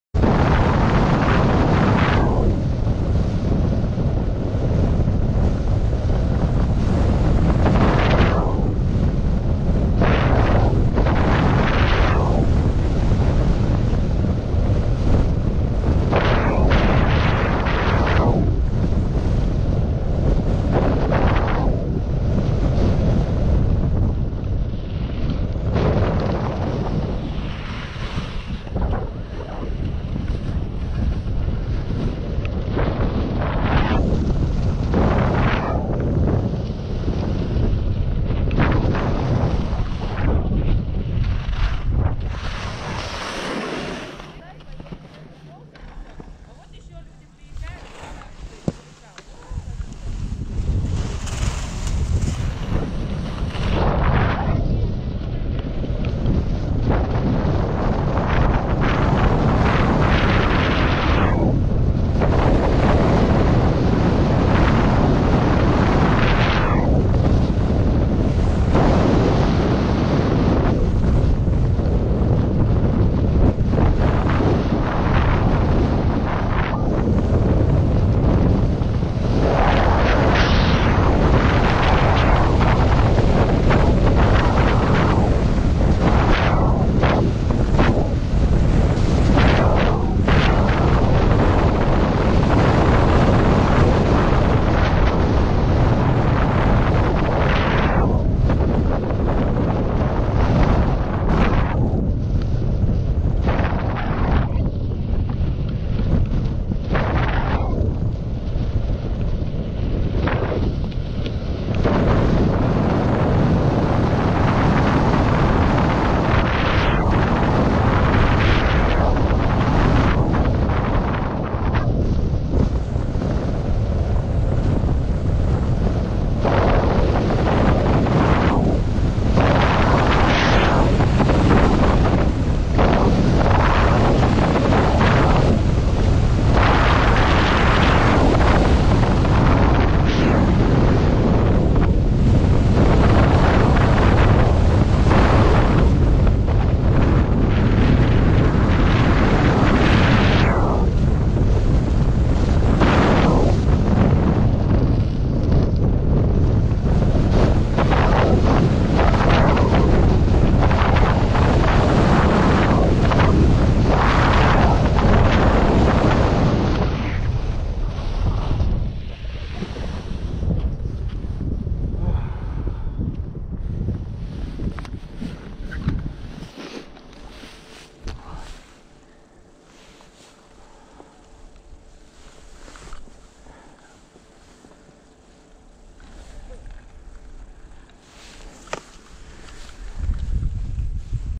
Video piste sci Corvara: da Crep du Munt Al planac
Ed ora ecco un altro bel video delle Video piste sci di Corvara: la discesa da Crep du Munt al Planac ripresa da Massimo Traverso che ringrazio molto come sempre.

Non perdetevi anche la nostra guida a Corvara con tanti altri video di piste a scoprire